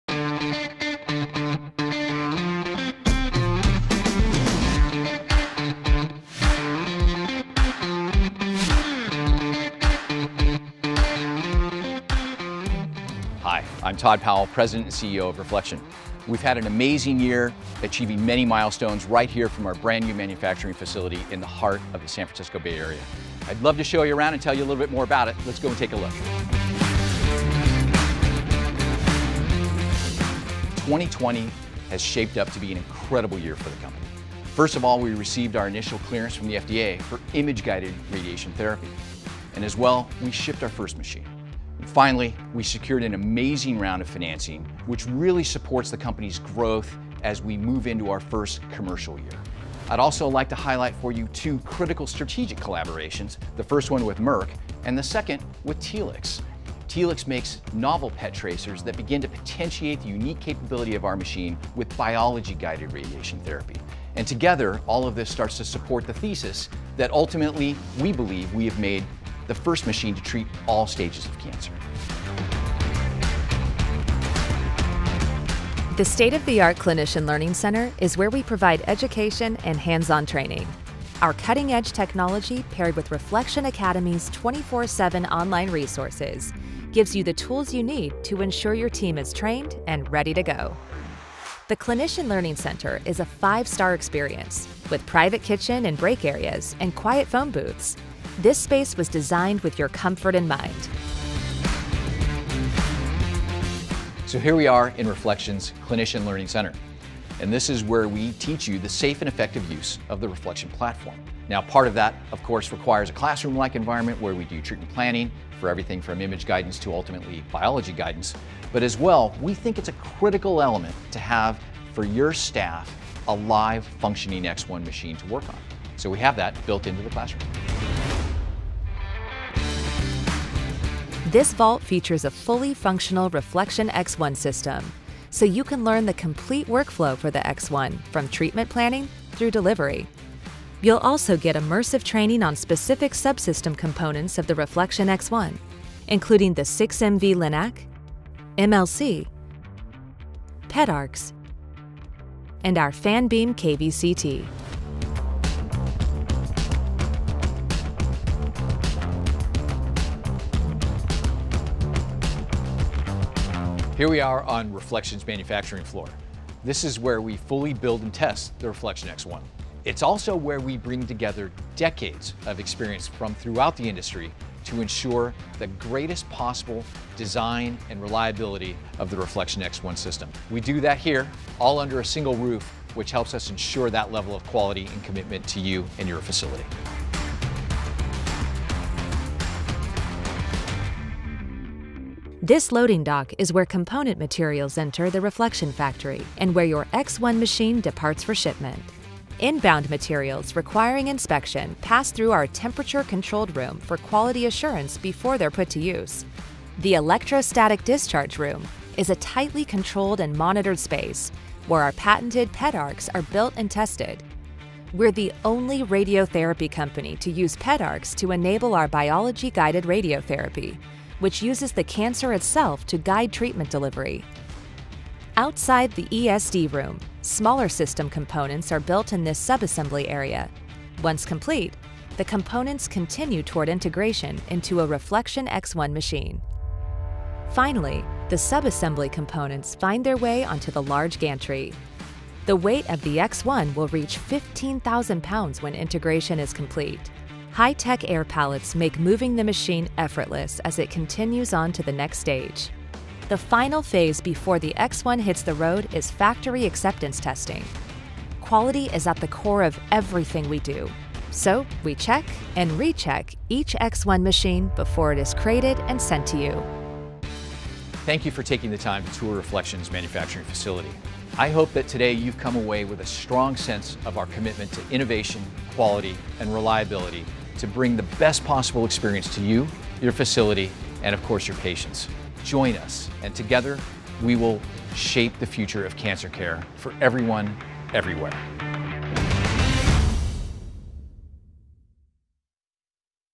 Virtual Tour: RefleXion HQ | Home of the X1
See where the RefleXion X1, our SBRT, SRS and IMRT machine—paving the way for biology-guided radiotherapy (BgRT) is made. We're proud to share this tour of our 100,000 square foot campus in Hayward, CA – just outside San Francisco.

The RefleXion® X1 is cleared for SBRT/SRS/IMRT. BgRT is pending regulatory review and is not commercially available.